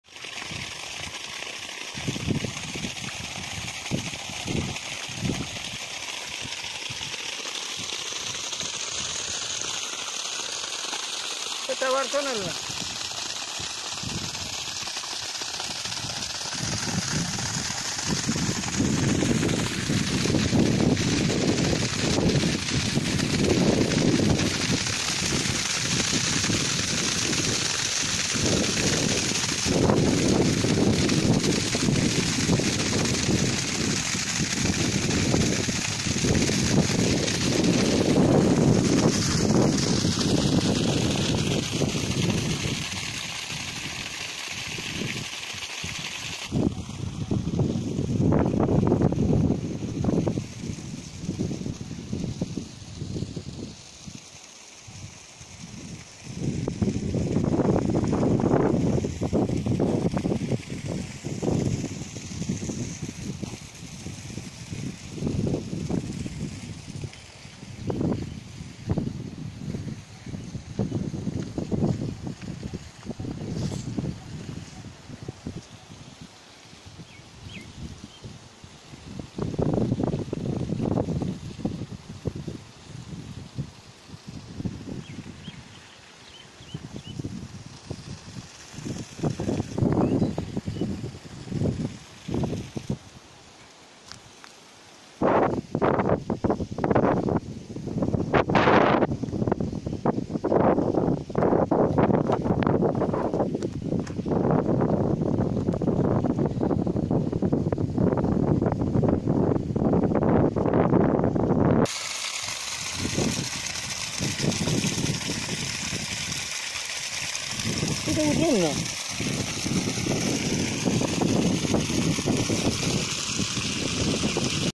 Commissioning of solar pump
Solar 3HP pump commissioning successfully.